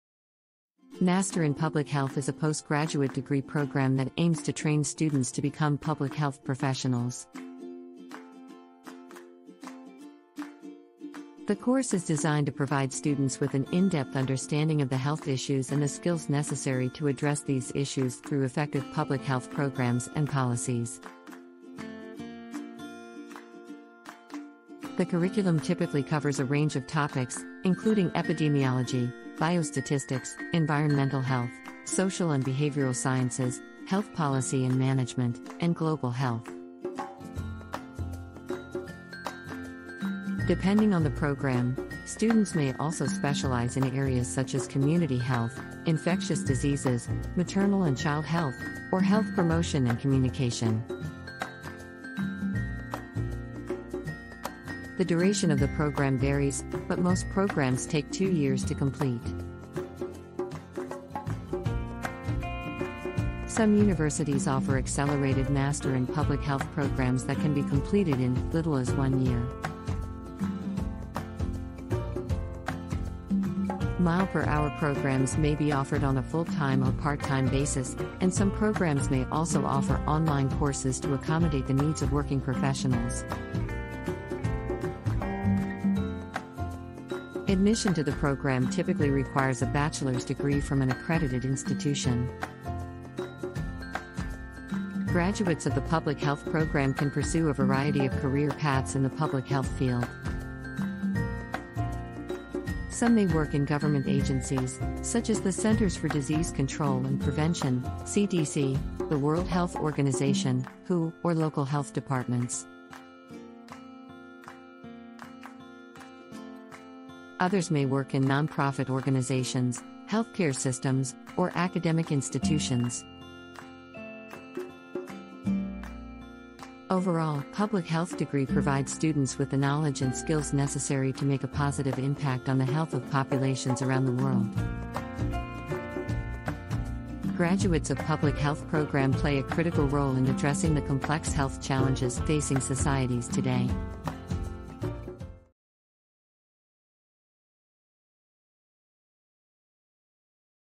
Master of Public Health (MPH)
What is the MPH course?
Master of Public Health (MPH) is a postgraduate degree program that aims to train students to become public health professionals. The course is designed to provide students with an in-depth understanding of the health issues facing populations, as well as the skills necessary to address these issues through effective public health programs and policies.
The MPH curriculum typically covers a range of topics, including epidemiology, biostatistics, environmental health, social and behavioral sciences, health policy and management, and global health. Depending on the program, students may also have the opportunity to specialize in areas such as community health, infectious diseases, maternal and child health, or health promotion and communication.

The duration of an MPH program varies, but most programs take two years to complete. Some universities offer accelerated MPH programs that can be completed in as little as one year. MPH programs may be offered on a full-time or part-time basis, and some programs may also offer online courses to accommodate the needs of working professionals.

Admission to an MPH program typically requires a bachelor's degree from an accredited institution, a competitive GPA, and letters of recommendation. Some programs may also require standardized test scores, such as the GRE or GMAT.

Graduates of MPH programs can pursue a variety of career paths in the public health field. Some may work in government agencies like the Centers for Disease Control and Prevention (CDC), the World Health Organization (WHO), or local health departments. Others may work in non-profit organizations, healthcare systems, or academic institutions.

Overall, an MPH degree provides students with the knowledge and skills necessary to positively impact the health of populations around the world. Graduates of MPH programs play a critical role in addressing the complex health challenges facing societies today.